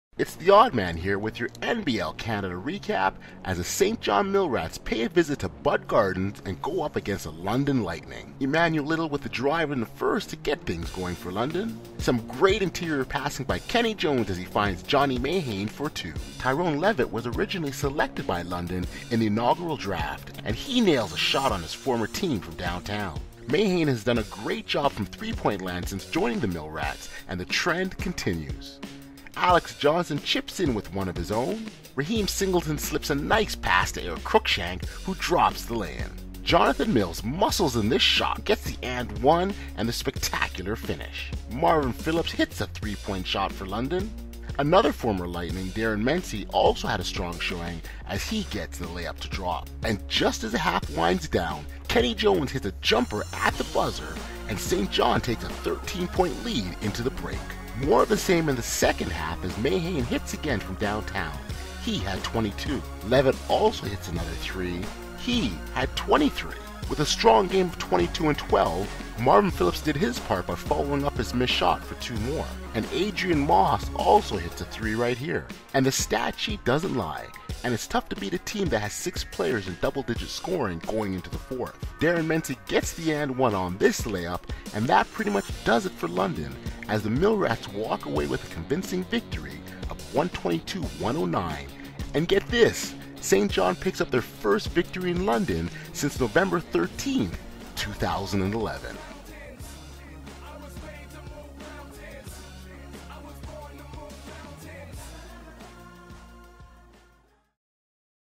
Saint John Mill Rats With Strong Bounce Back vs London Lightning
Game Recap   London Lightning vs Saint John Mill Rats Jan 11 2015

The Saint John Mill Rats bounced back in a big way in front of a record crowd, 122-109, over the London Lightning.

The Mill Rats were without the services of League MVP Anthony Anderson, who was injured the night before in Windsor, but that didn't stop them from a huge road win in front of an NBL Canada record 8,636 fans

Newly acquired guard Darin Mency filled in admirably with 28 big points.

Saint John led from wire to wire, building a lead as big as 21 points, and London never got within less than 10 in the fourth quarter.

"We put together a nearly complete game today, and now the guys are buying into doing the right things at the right time," said Mill Rats Head Coach Julian King. "Without Double-A everyone wanted to step up and contribute and the results were that we came together and played as a team."

The Mill Rats also were without 6'8 power forward Ricky Volcy who is still awaiting FIBA clearance.

In addition to Mency's 28, Tyrone Levett scored 23, Johnny Mayhane 22, Kenny Jones 21, Raheem Singleton 14, and Alex Johnson 10.

The Mill Rats improve to 8-13 on the season while the Lightning drops to 11-8.